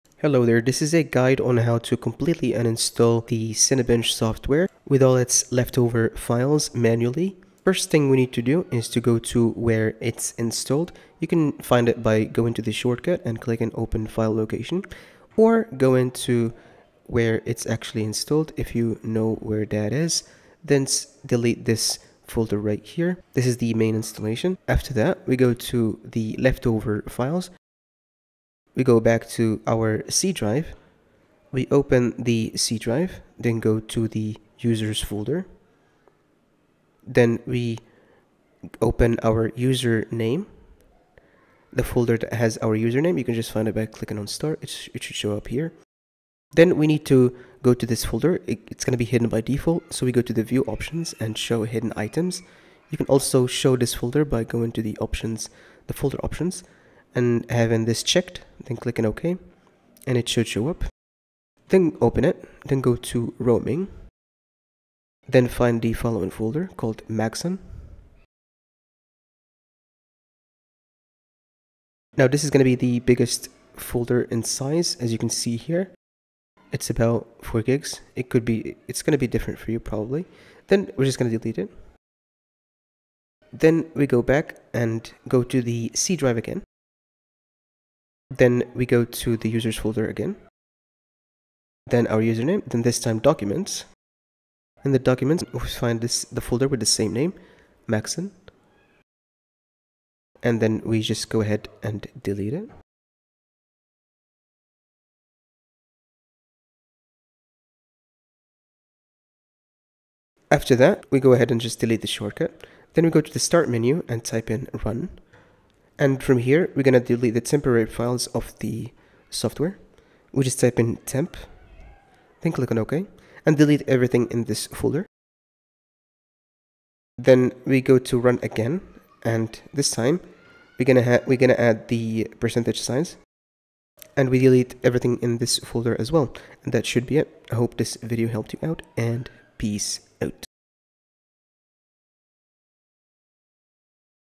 How To Completely Uninstall Cinebench On Windows | Save DiskSpace
Guide on how to completely remove uninstall and cleanup the benchmarking software cinebench r23 the free as well as the paid versions saving you a considerable disk space size.

Send a tip as little as 2$ to support me and the channel